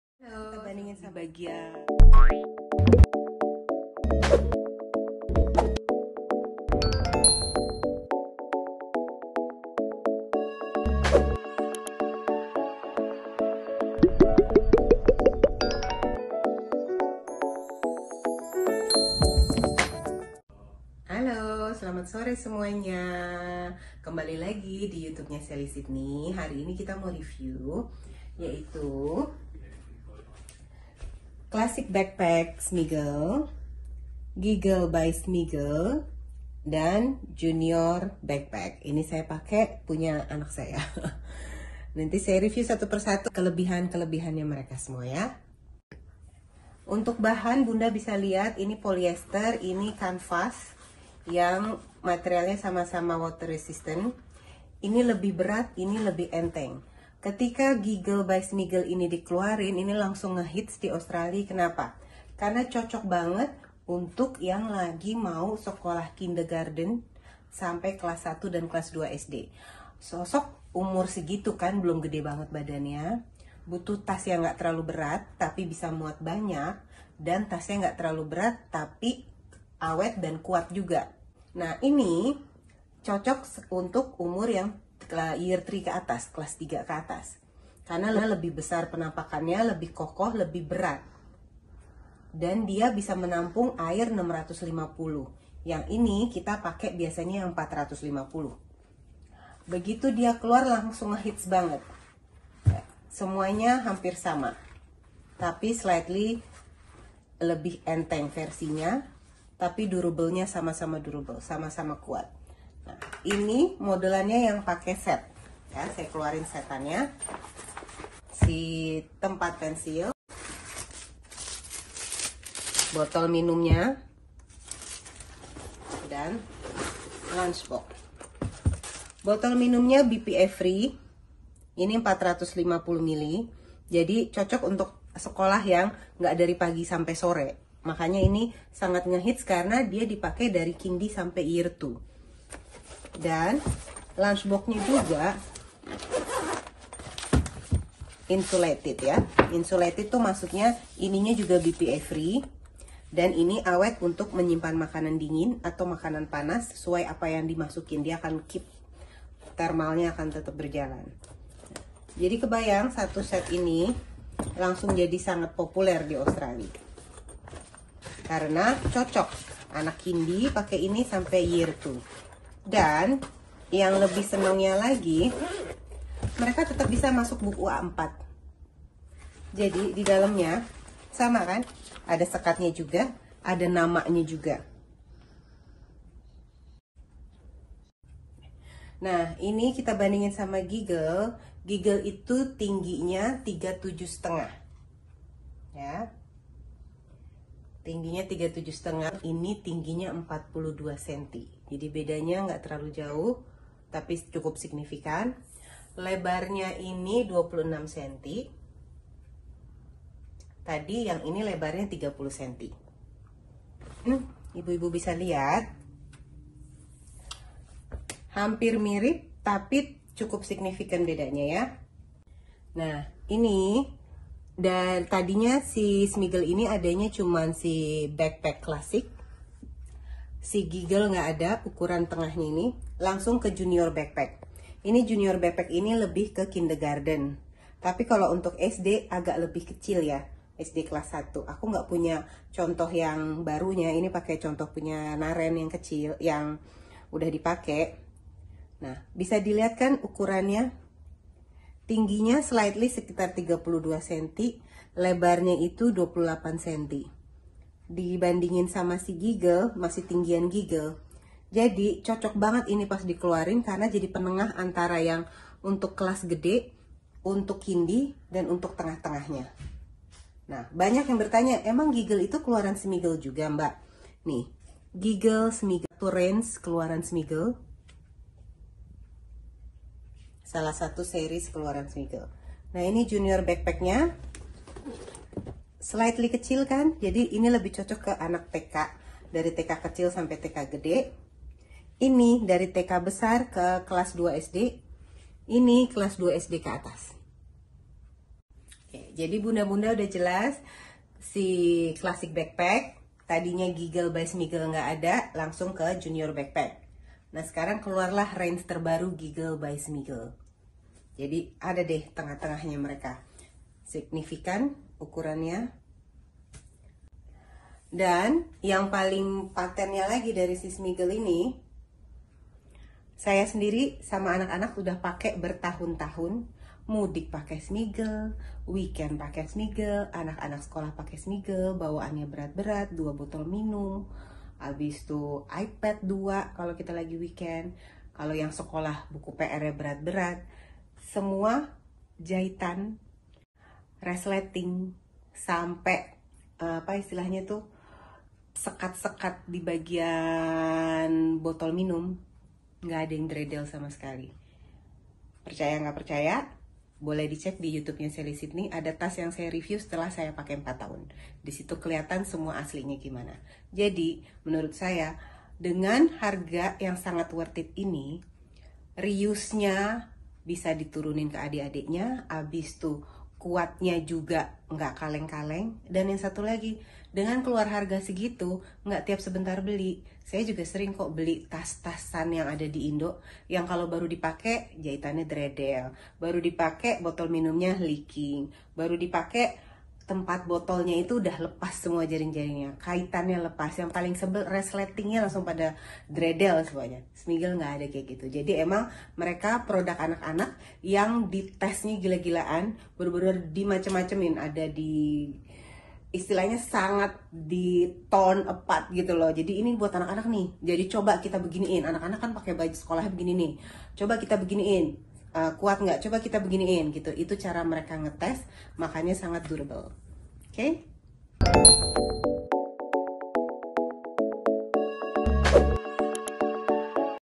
GIGGLE ITU KELUARAN SMIGGLE BUKAN SIH??!! | BACKPACK BEST SELLER DI AUSTRALIA!! SUPER WORTH IT
untuk yg mau request bisa comment di vidio ini yaa

dan jangan luoa follow instagram kita